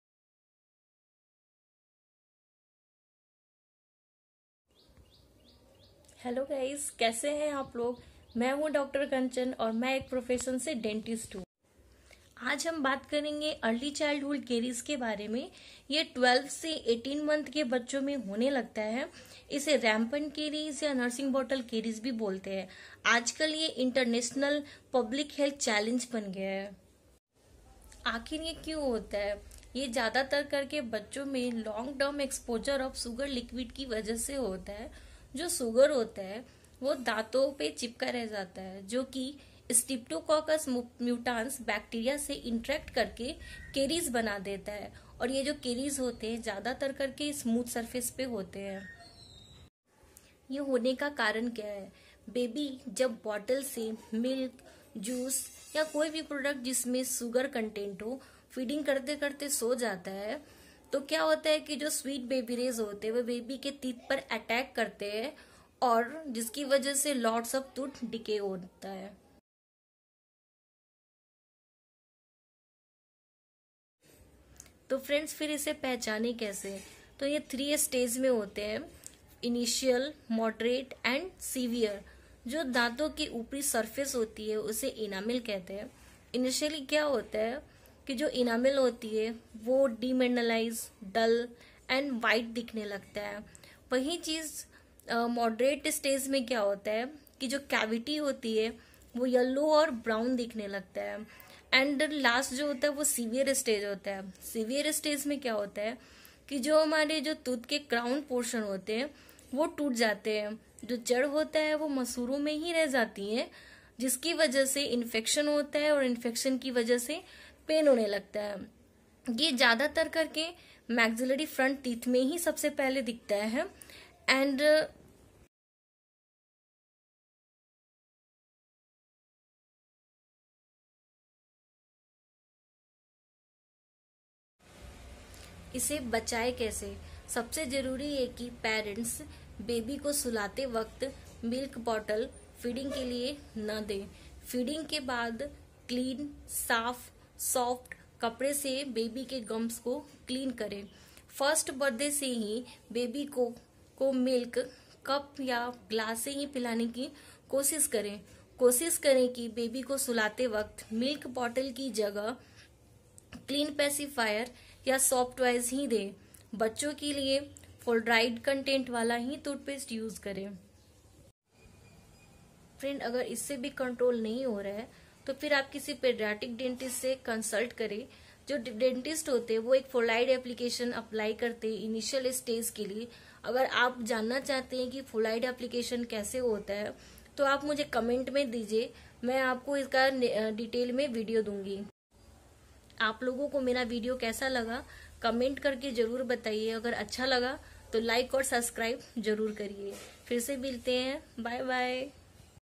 Prevention of Early Childhood Caries/बच्चों के दाँतो को सडने से कैसे बचाये
How to prevent Early caries in child